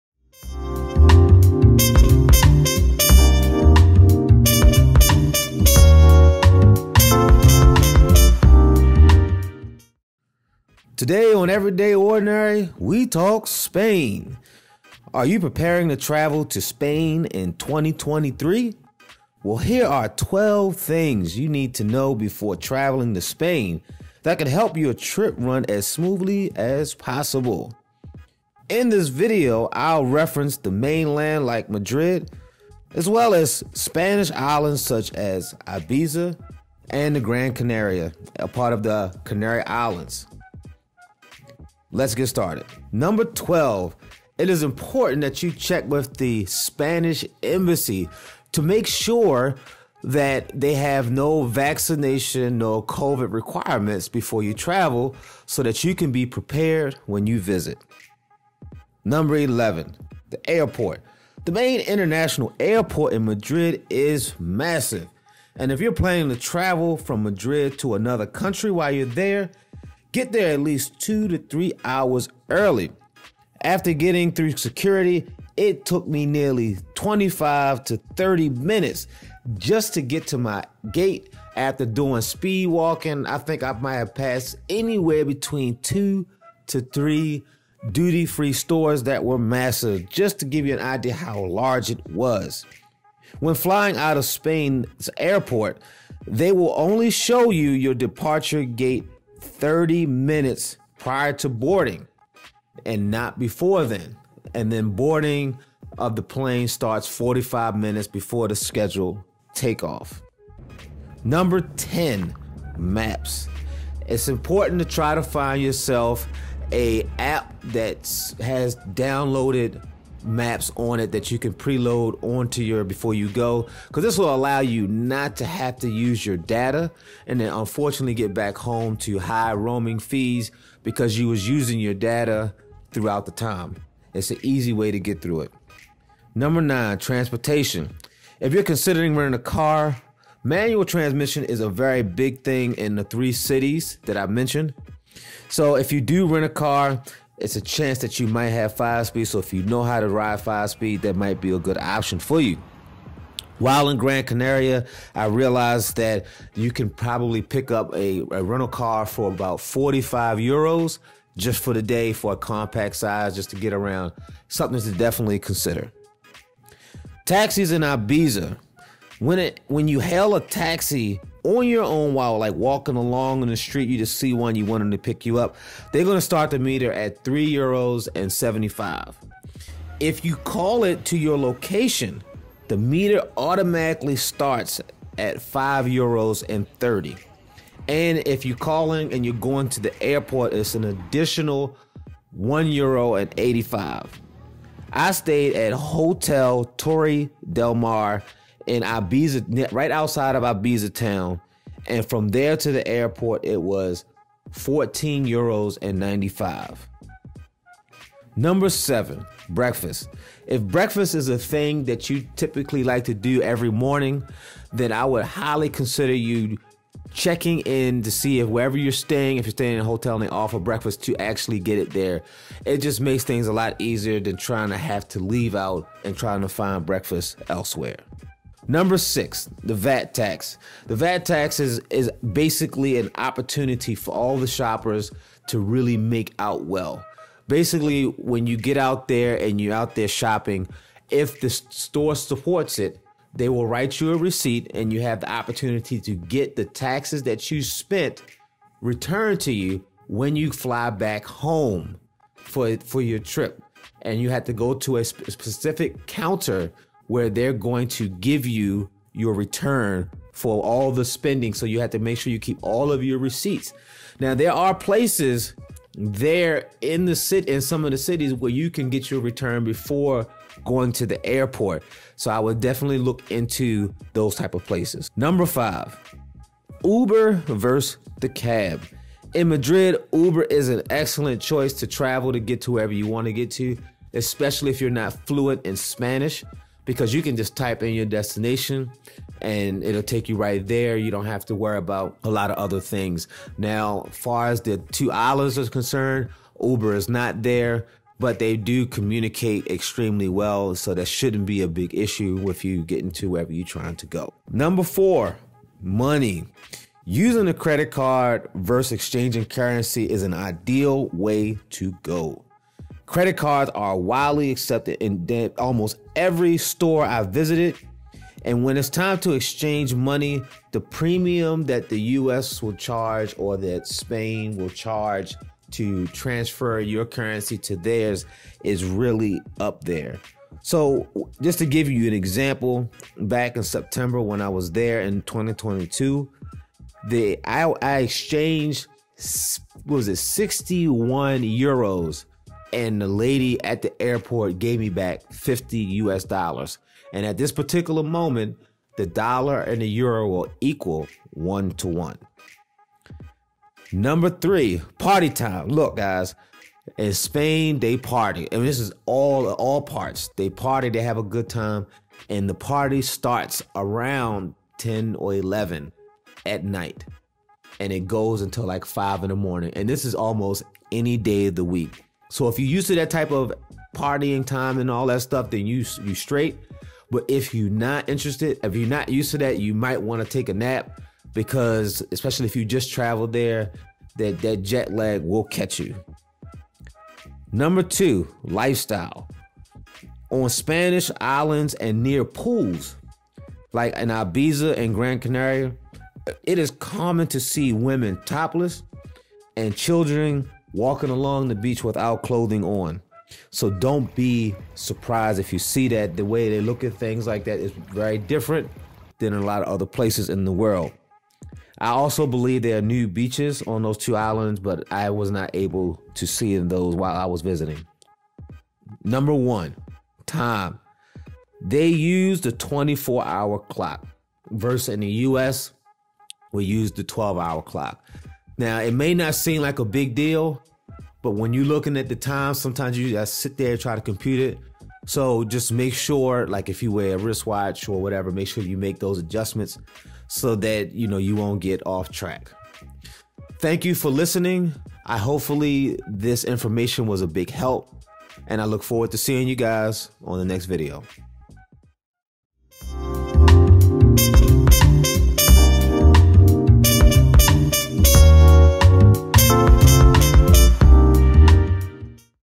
Planning to visit SPAIN??? 12 THINGS YOU NEED TO KNOW 2023
Within this video, 12 concepts will be shared that would put you in a better position, if you’re planning to travel to Spain in 2023 and beyond. The locations of Spain featured in this presentation is Madrid, Gran Canaria of the Canary Islands and the island of Ibiza.